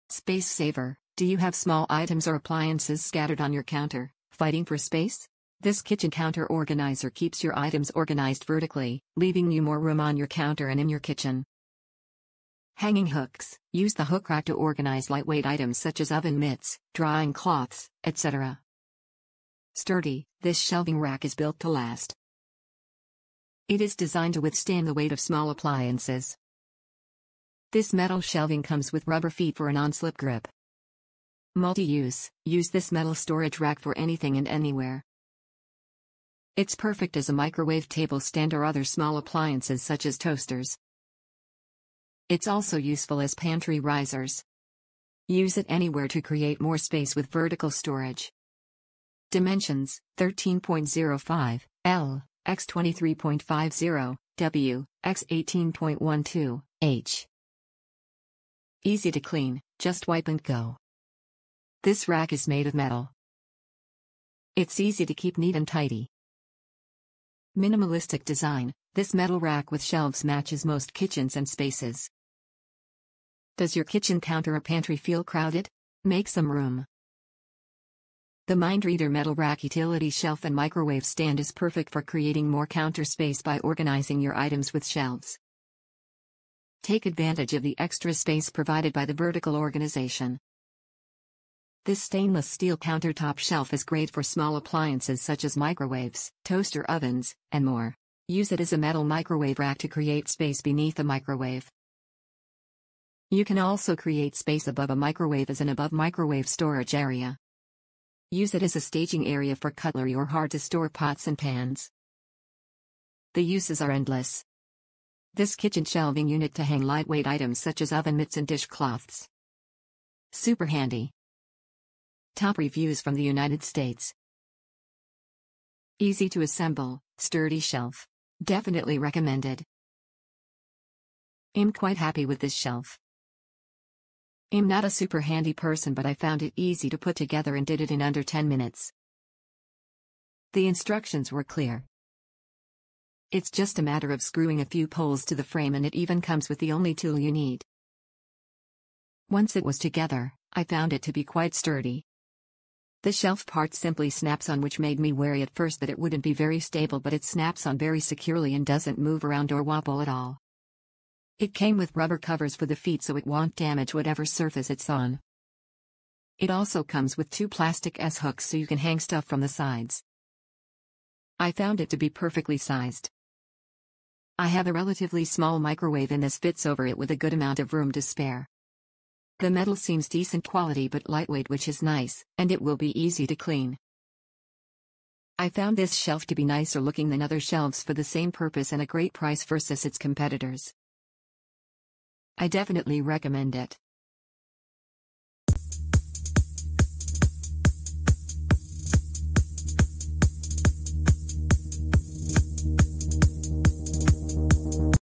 Mind Reader Microwave Oven Rack Shelf Unit for Kitchen Utensils, Towels, Mits, and More, Black
SPACE-SAVER: Do you have small items or appliances scattered on your counter, fighting for space? This kitchen counter organizer keeps your items organized vertically, leaving you more room on your counter and in your kitchen.
HANGING HOOKS: Use the hook rack to organize lightweight items such as oven mitts, drying cloths, etc.
STURDY: This shelving rack is built to last. It is designed to withstand the weight of small appliances. This metal shelving comes with rubber feet for a non-slip grip.
MULTI-USE: Use this metal storage rack for anything and anywhere. It’s perfect as a microwave table stand or other small appliances such as toasters. It’s also useful as pantry risers. Use it anywhere to create more space with vertical storage.
EASY TO CLEAN: Just wipe and go. This rack is made of metal. It’s easy to keep neat and tidy.
MINIMALISTIC DESIGN: This metal rack with shelves matches most kitchens and spaces.
"Does your kitchen counter or pantry feel crowded? Make some room!
The MIND READER Metal Rack Utility Shelf and Microwave Stand is perfect for creating more counter space by organizing your items with shelves. Take advantage of the extra space provided by the vertical organization.
This stainless-steel countertop shelf is great for small appliances such as microwaves, toaster ovens, and more! Use it as a metal microwave rack to create space beneath a microwave. You can also create space above a microwave as an above microwave storage area. Use it as a staging area for cutlery or hard to store pots and pans. The uses are endless!
This kitchen shelving unit to hang lightweight items such as oven mitts and dish cloths. Super handy!

Easy to assemble, sturdy shelf! Definitely recommended
I'm quite happy with this shelf. I'm not a super handy person but I found it easy to put together and did it in under 10 minutes. The instructions were clear. It's just a matter of screwing a few poles to the frame and it even comes with the only tool you need.

Once it was together, I found it to be quite sturdy. The shelf part simply snaps on which made me wary at first that it wouldn't be very stable but it snaps on very securely and doesn't move around or wobble at all.

It came with rubber covers for the feet so it won't damage whatever surface it's on. It also comes with two plastic s-hooks so you can hang stuff from the sides.

I found it to be perfectly sized. I have a relatively small microwave and this fits over it with a good amount of room to spare. The metal seems decent quality but lightweight which is nice, and it will be easy to clean.

I found this shelf to be nicer looking than other shelves for the same purpose and a great price versus its competitors. I definitely recommend it.